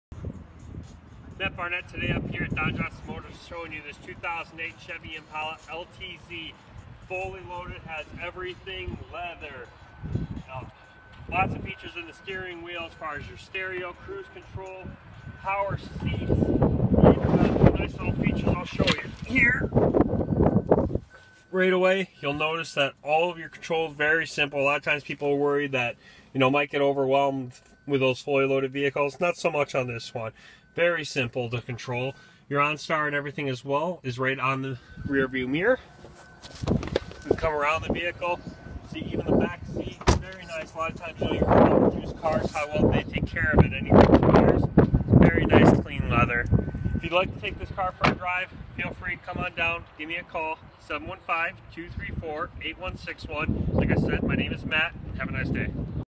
2008 Chevrolet Impala at Don Johnson Motors in Rice Lake, WI
Matt Barnett, Product Expert at Don Johnson Motors in Rice Lake, WI, shares this 2008 Chevrolet Impala which is in great shape.